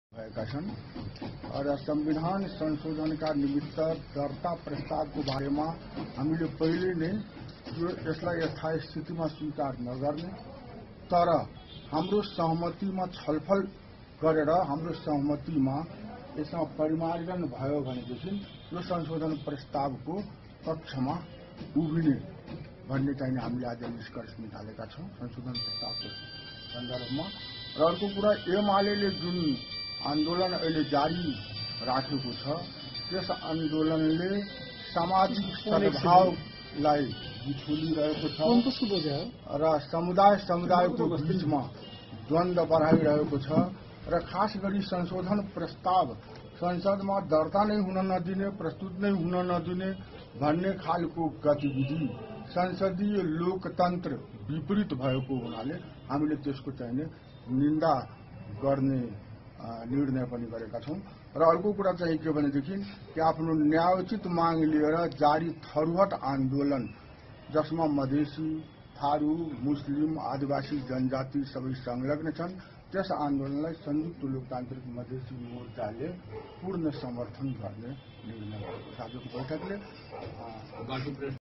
madheshi morcha leader brikshesh chandra lal
संशोधन प्रस्ताव पारित गराउन मधेशी मोर्चा लचिलो